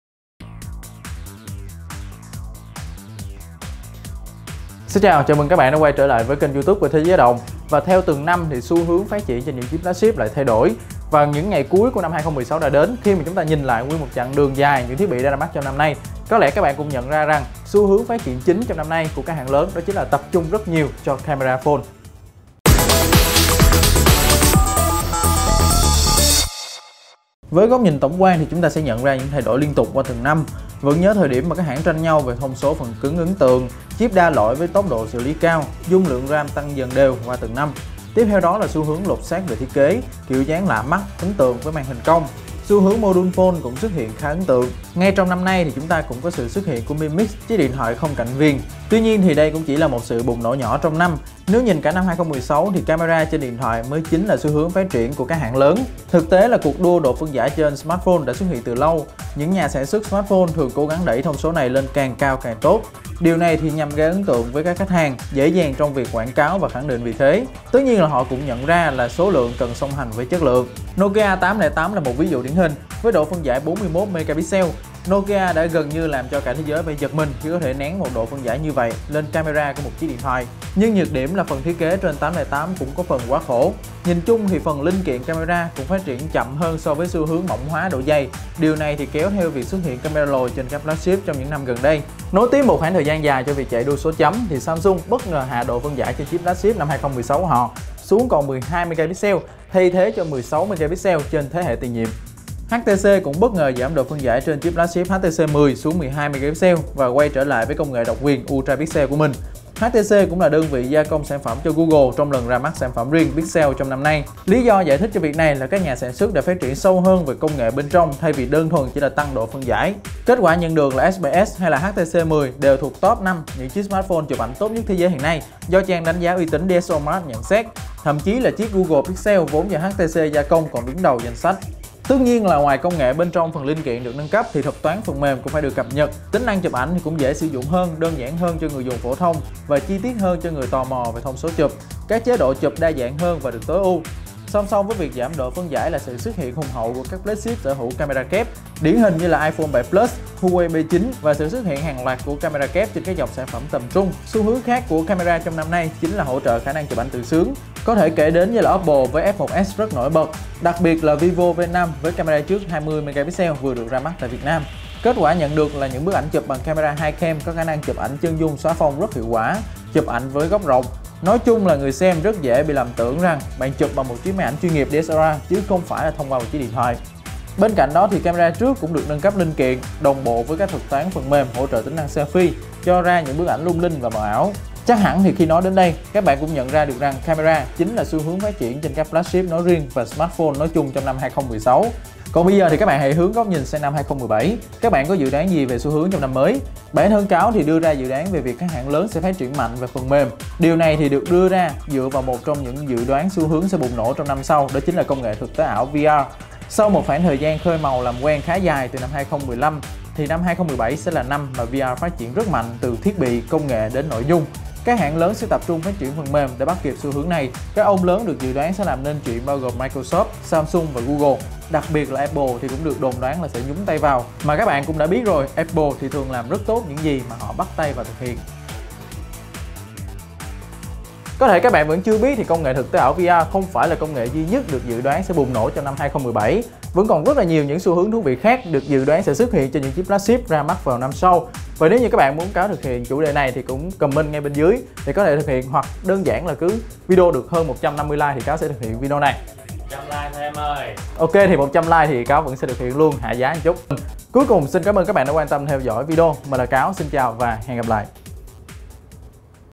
Tìm hiểu về xu hướng trên các Flagship 2016
Có thể bạn chưa biết? Xu hướng xuất hiện trên các flagship thường thay đổi qua từng năm, từ cấu hình đến thiết kế v.v... Bạn đã nhận ra xu hướng trên các flagship 2016 là gì chưa?! Hoặc bạn dự đoán trên flagship 2017 là gì?! Hãy cùng xem video để biết thêm chi tiết (và cùng chém gió nhé!)...